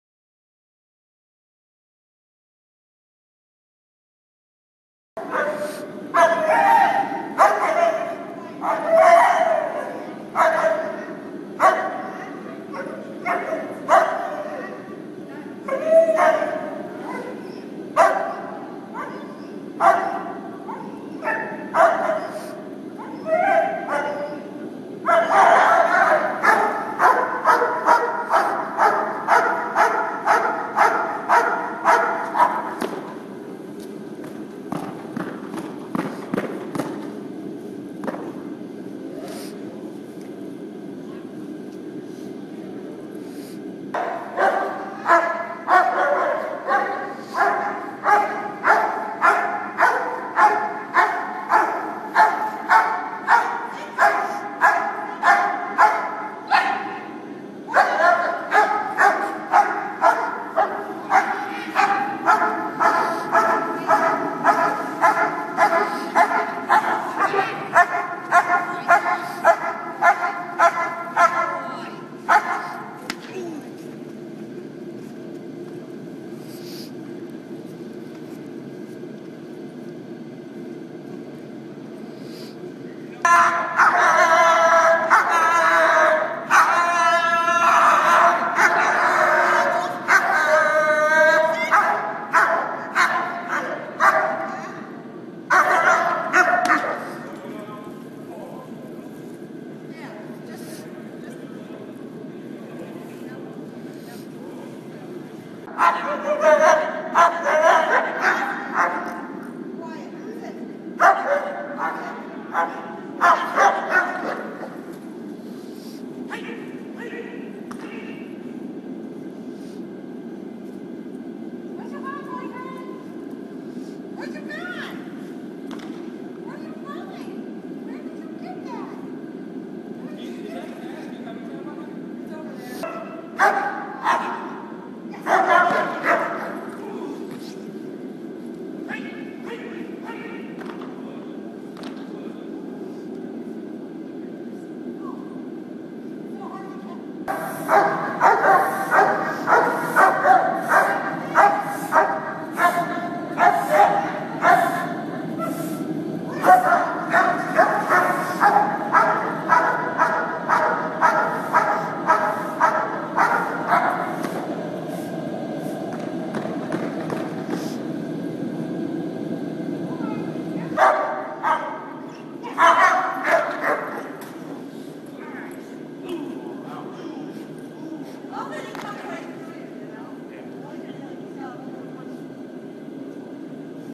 Dasko 1st set protection 9Jan11
Dasko, 13 months old. trying to work on him being a little quieter in protection... it's a slow process. the boy likes to make noise.

Helper is Kevin Girvin